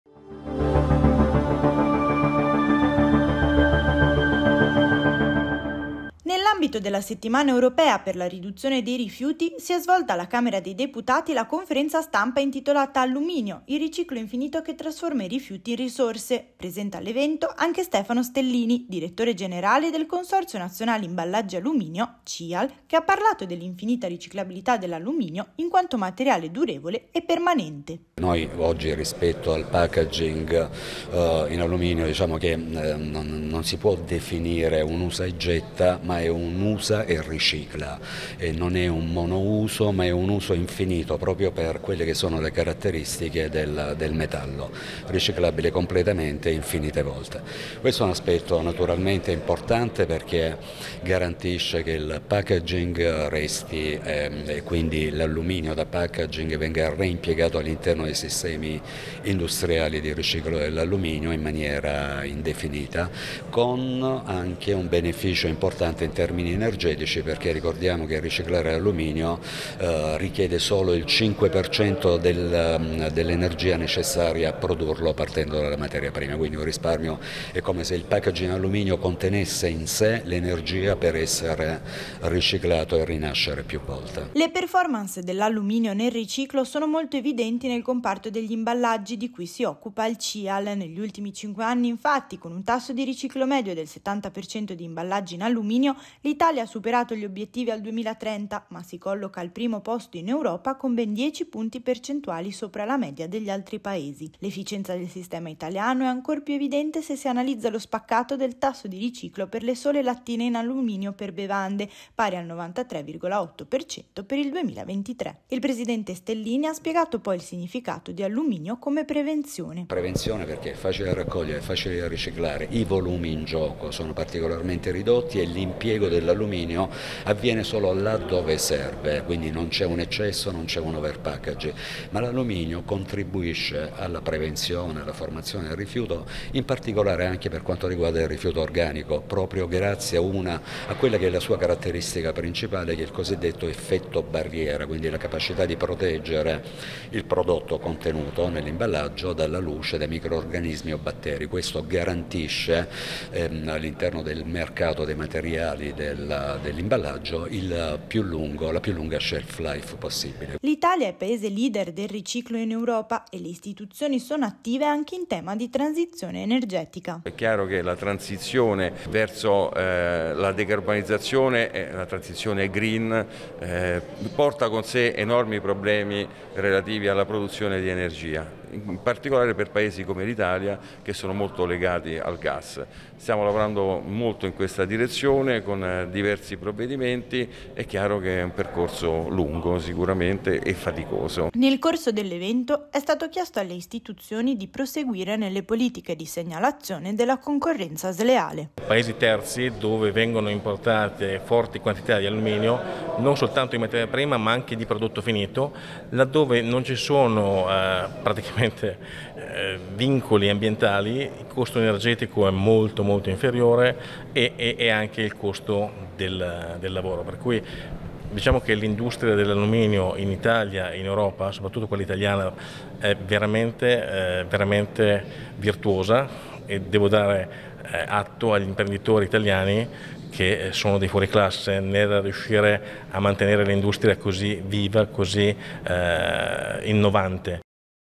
Economia circolare, riciclo alluminio è preziosa risorsa italiana
‘Alluminio: il riciclo infinito che trasforma i rifiuti in risorse’. E’ questo il titolo della conferenza stampa organizzata alla Camera dei Deputati a Roma, nell’ambito della settimana europea per la riduzione dei rifiuti. Presenti all’evento illustri personalità del settore che hanno discusso sul tema, tra cui anche il direttore generale del Consorzio Nazionale Imballaggi Alluminio - CiAL, Stefano Stellini.